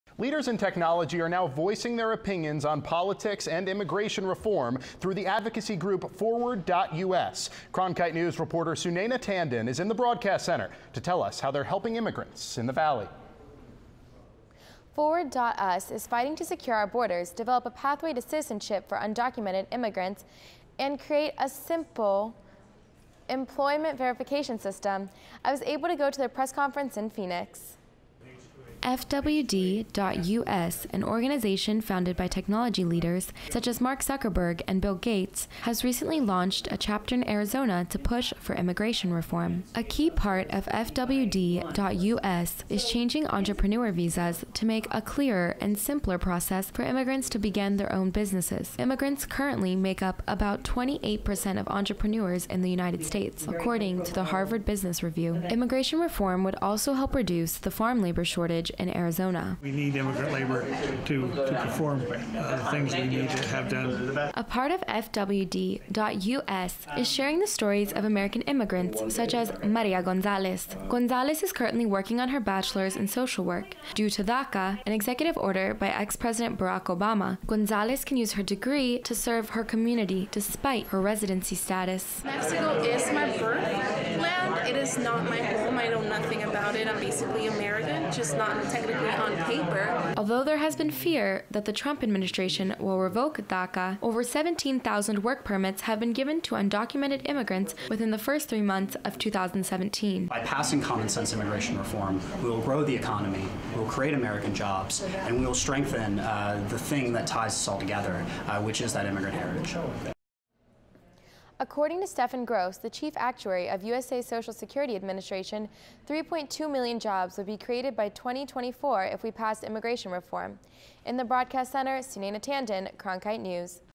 FWD.us launches Arizona chapter to push for immigration reform | Cronkite News
The organization aims to develop a pathway to citizenship for undocumented immigrants and create a simple employment verification system for immigrants to start their own businesses.(Video by Sunaina Tandon/Cronkite News)